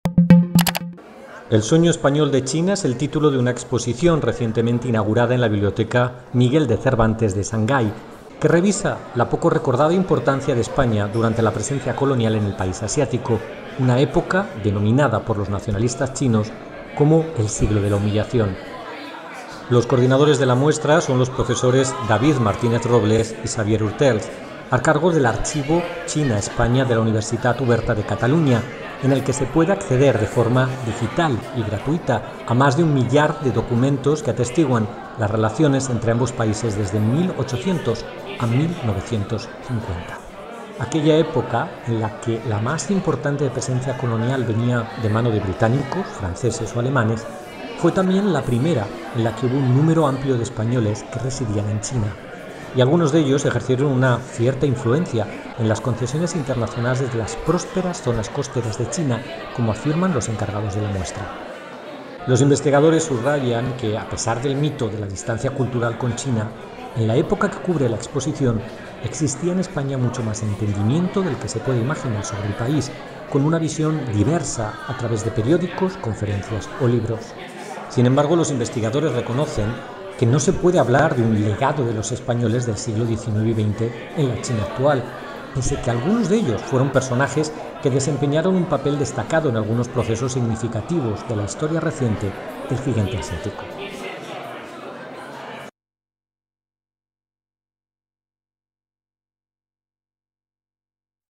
El "sueño" de los pioneros de las relaciones entre España y China
Shanghái (China), 21 oct (EFE).- Sinibaldo de Mas, Antonio Ramos o Juan Mencarini son nombres olvidados por la mayoría de españoles, pero en la segunda mitad del siglo XIX y la primera del XX fueron los pioneros de uno de los lazos diplomáticos de más relevancia en la actualidad para el país ibérico, los que lo unen con China.
Imagen: Víctor Escribano